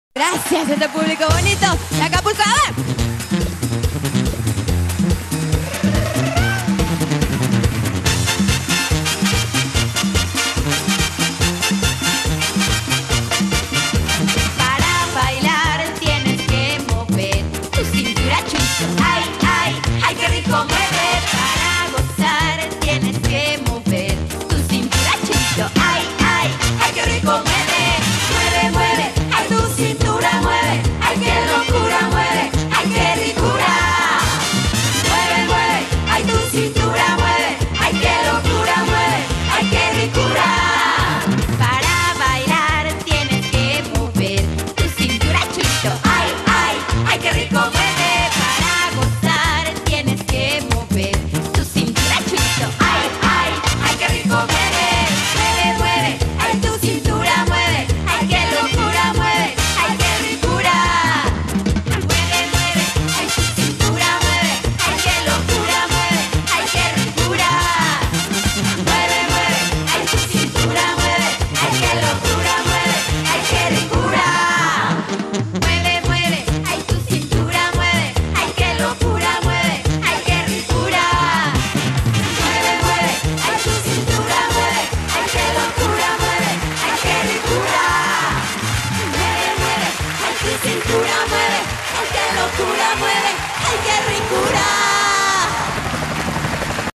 tromba sinaloense
la tromba sinaloense con luz elena gonzalez en el acafest 2004